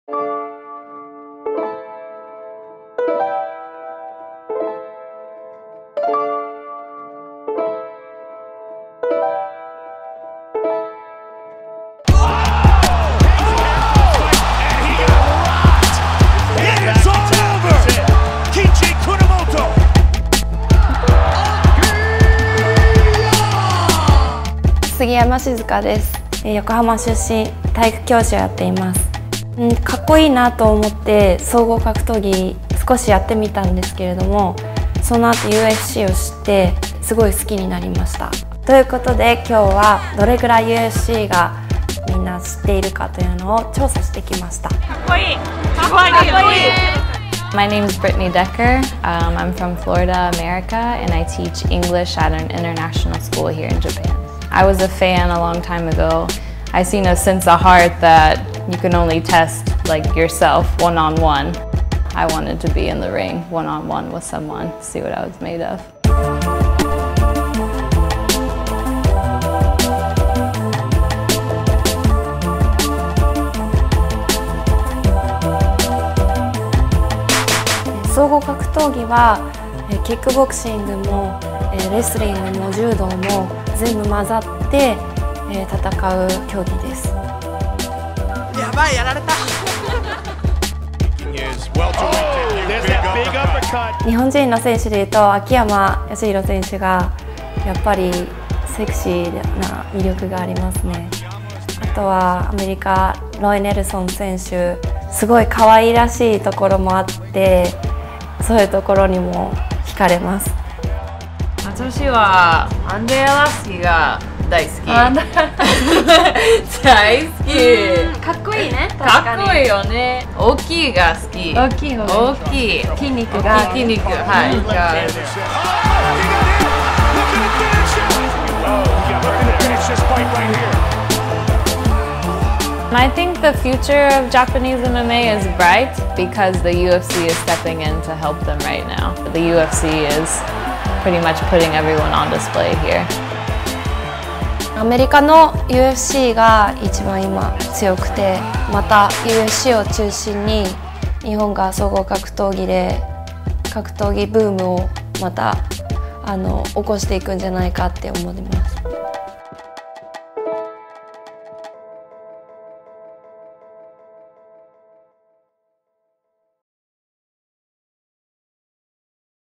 An Appetite for Strength: The Rebirth of Japanese MMA
PRIDE used to be the face of MMA in Japan. When that promotion dissolved, it left a void in a market that only a few years ago had been thriving.  Fortunately, the UFC has now emerged as a dominant force in the East, giving millions of eager fans the opportunity to once again watch the toughest fighters in the world go head-to-head.  While this rebirth of MMA in Japan is new to some, local fight fans have long been plugged in to UFC events via their cell phones or cable TV.  Fan momentum has lead to the widely-publicized heavyweight headlining match between Mark Hunt and Roy Nelson to be held at UFC Fight Night on September 20 at Saitama Super Arena, a massive 20,000-seat arena in the heart of Japan. VICE follows UFC fans Shizuka Sugiyama and Brittany Decker to see how popular UFC is in Tokyo.